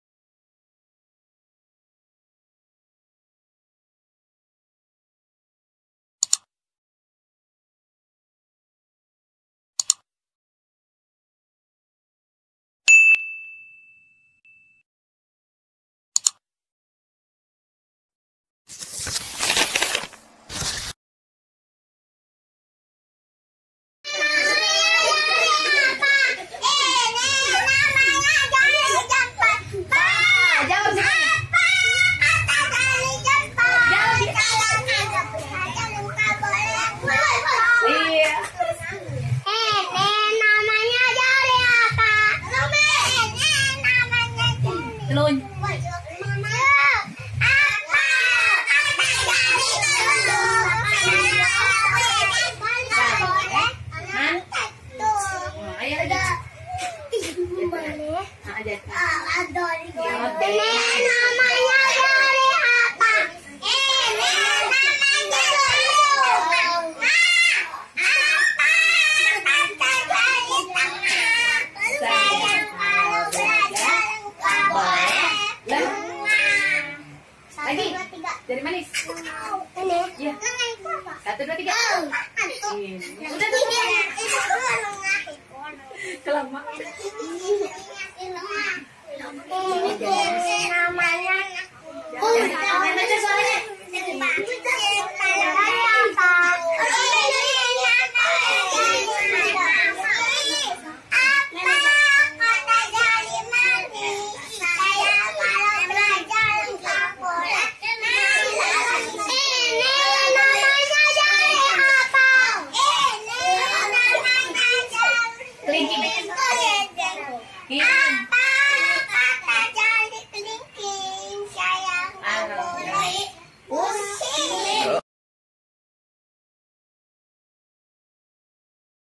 Keseruan Anak PAUD Di Saat Mengenalkan Nama-Nama Jari Tangan Sambil Bernyanyi
Chenel Ini Buat Pengenalan Untuk Anak-Anak Agar Cepat Mengingat Nama-Nama Jari Tangan.
Semoga Bermanfaat.Amiiin